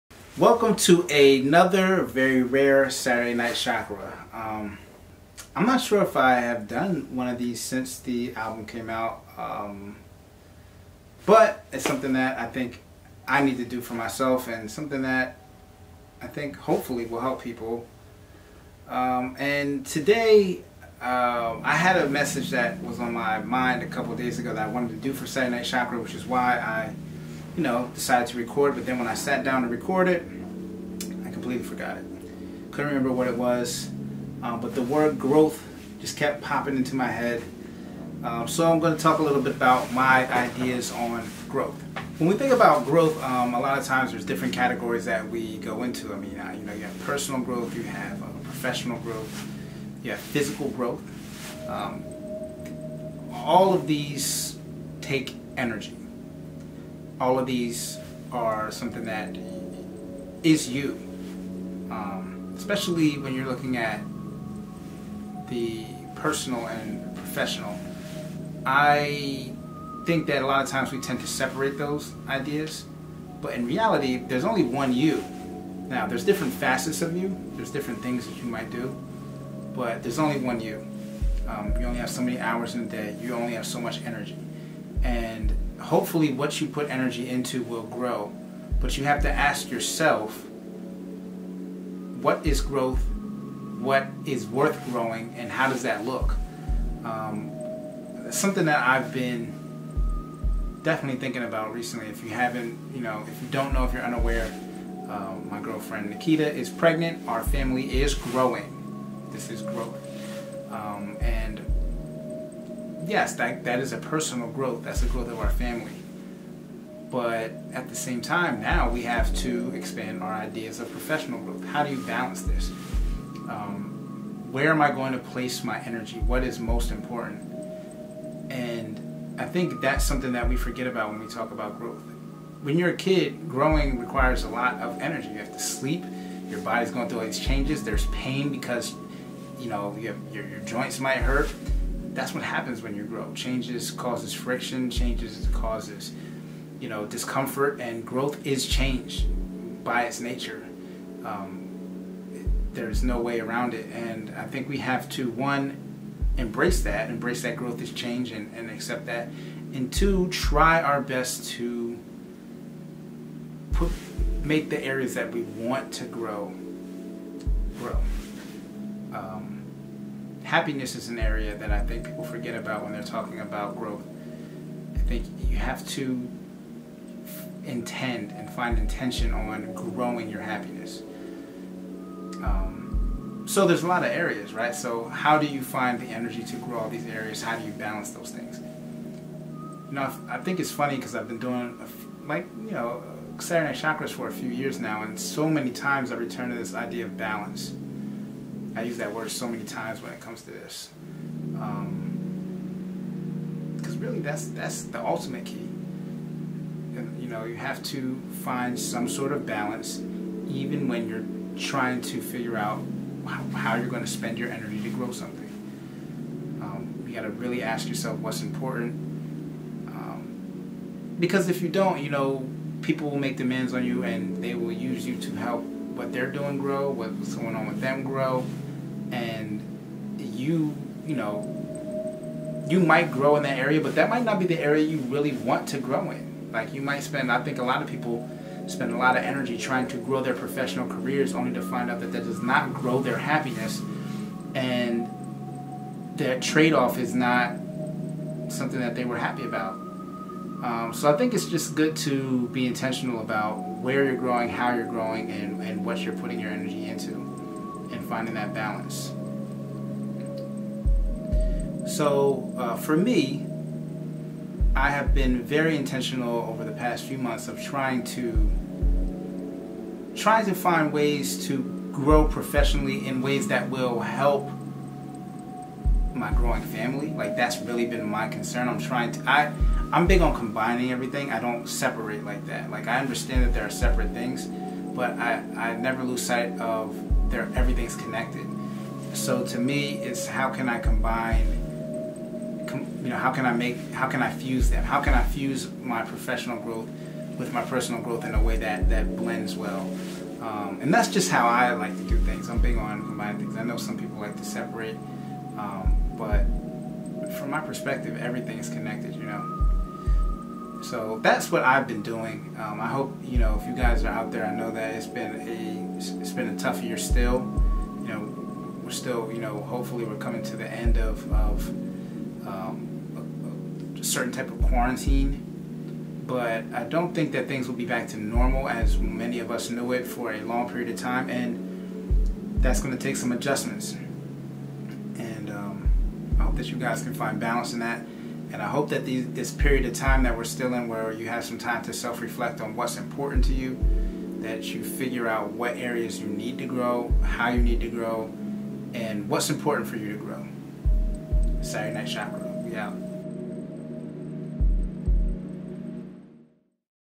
What does "Growth" mean? -Saturday Night Chakra
Its been a while since I did a Saturday Night Chakra.  I went into this one with a fully fledged idea of what I wanted to say but then I completely forgot.
As I was sitting there trying to figure out if I wanted to do it the word growth kept popping in my head so I decided to make the Saturday Night Chakra about growth.